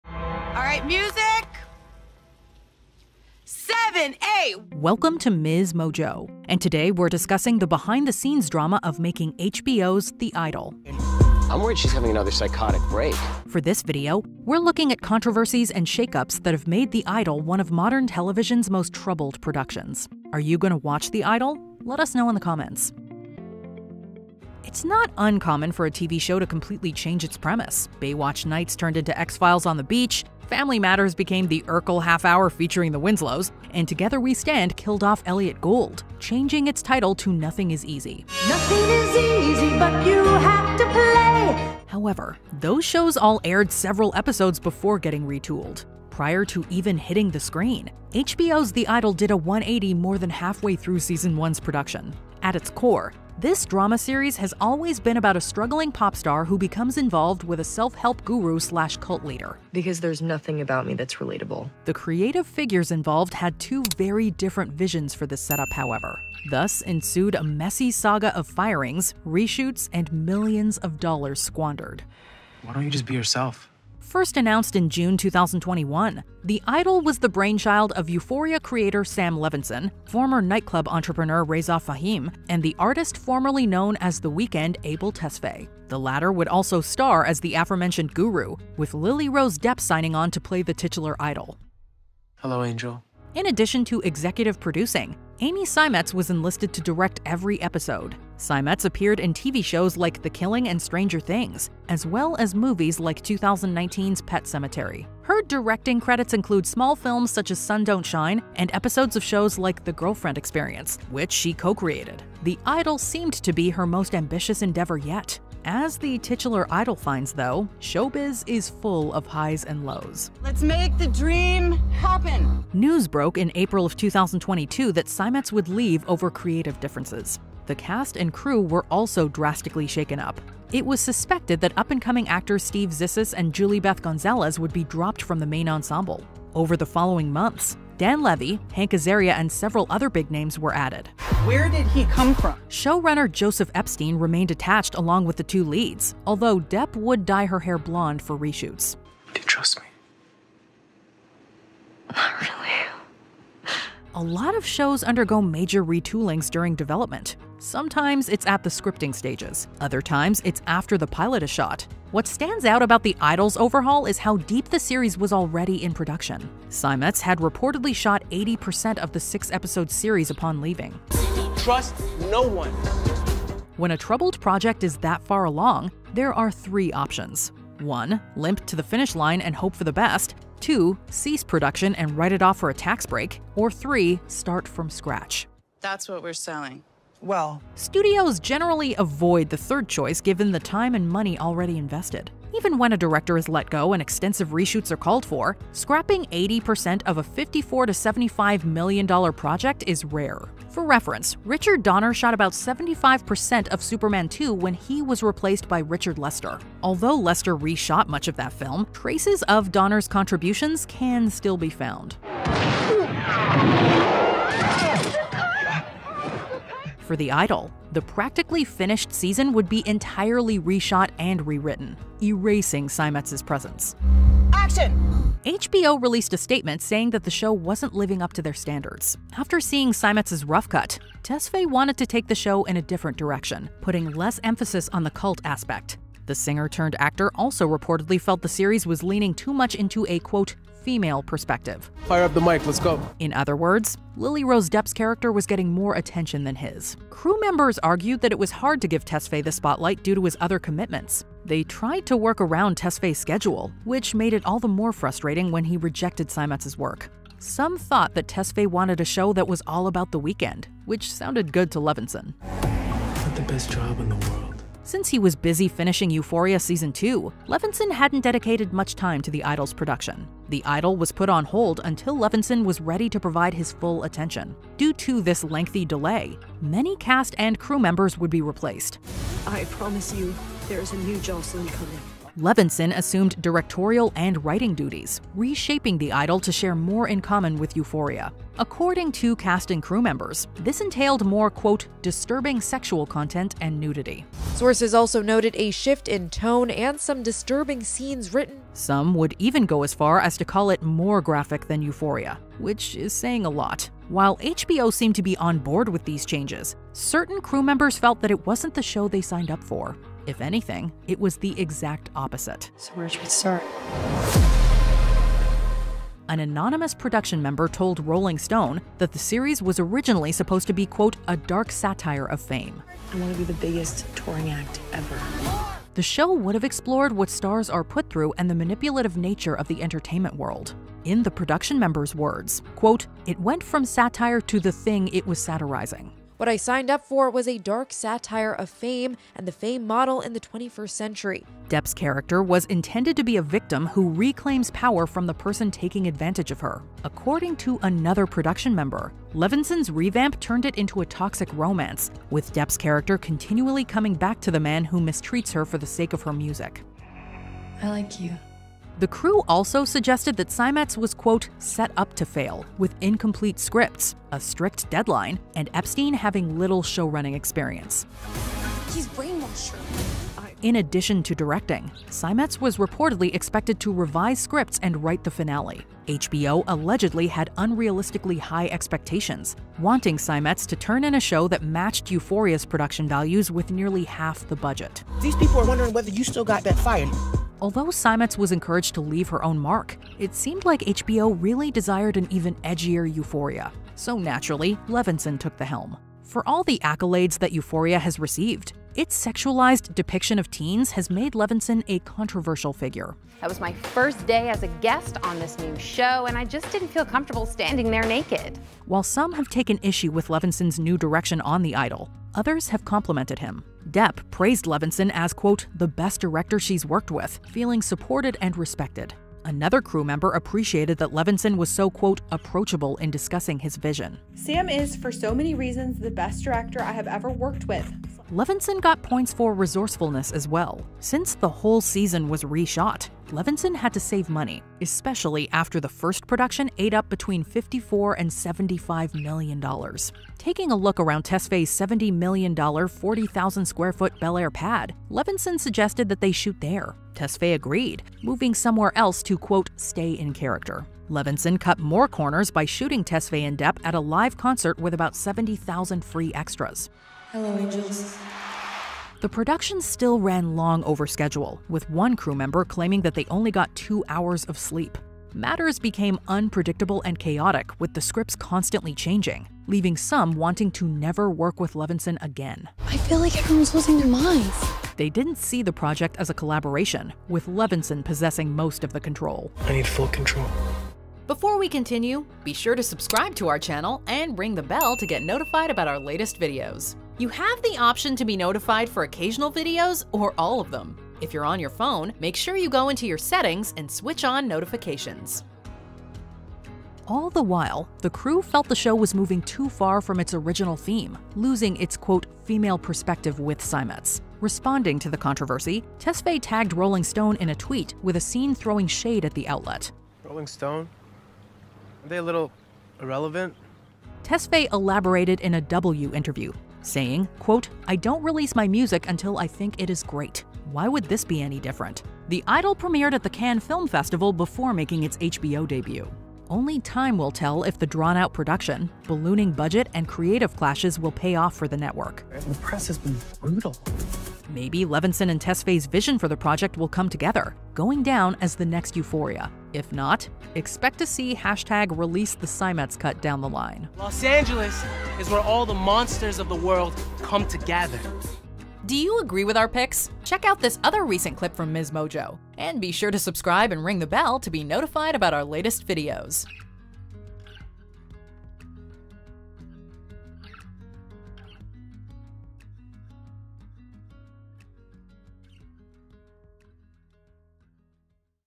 HBO's The Idol: The Behind the Scenes Drama of Making the Show
"The Idol" has caused a lot of drama before even airing. For this list, we’ll be looking at controversies and shake-ups that have made “The Idol” one of modern television’s most troubled productions. Our video includes Lily-Rose Depp, Abel Tesfaye, Sam Levinson, and more! Are YOU going to watch “The Idol?”